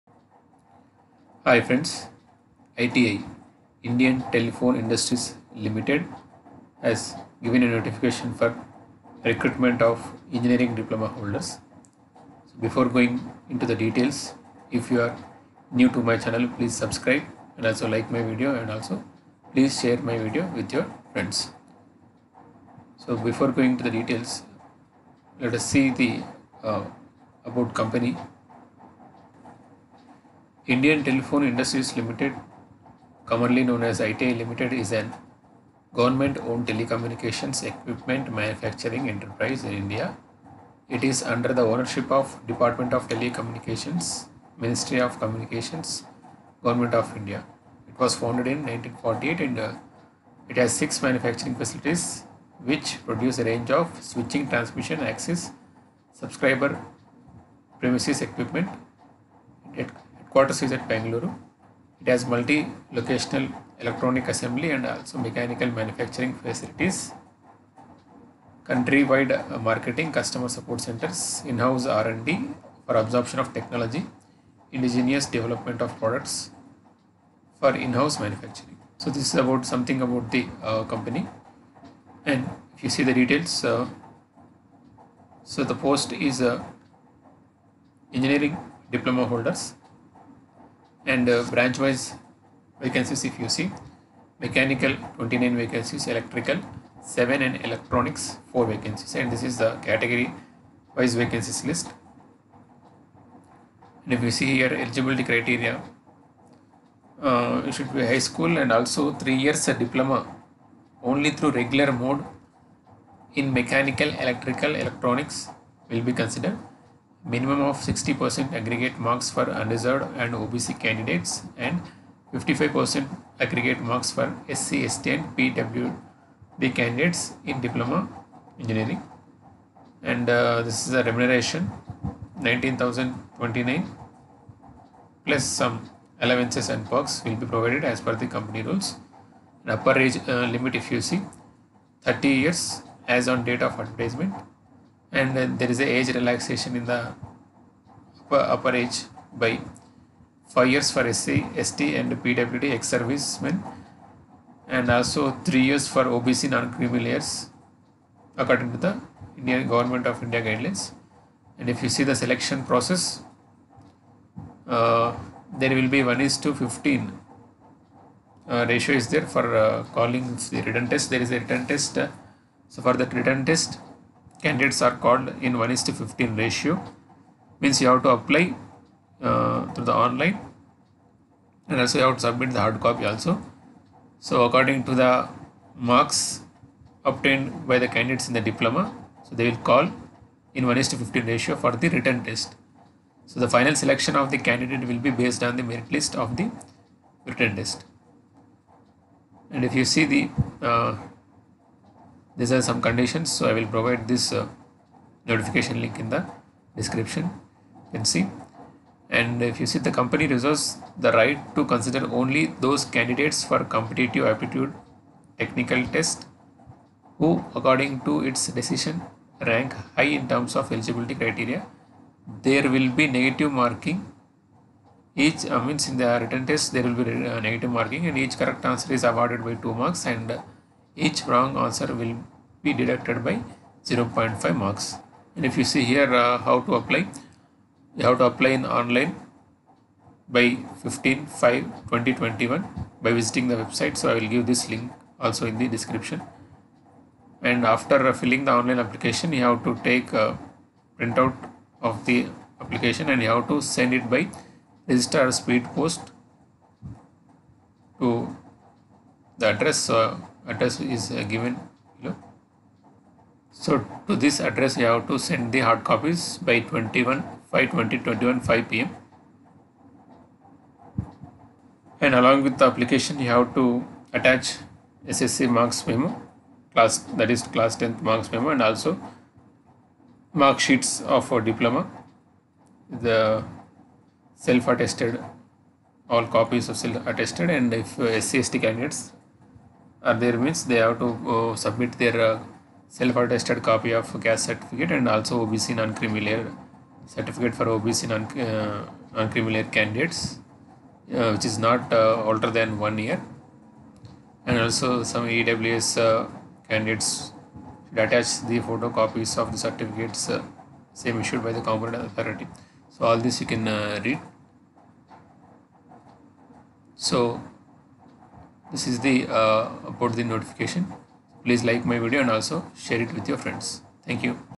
ITI ( Indian Telephone Industries Limited ) RECRUITMENT-2021, NO FEE , LATEST GOVT JOBS
ITI ( Indian Telephone Industries Limited ) RECRUITMENT-2021, NO FEE , LATEST GOVT JOBS, Freshers can Apply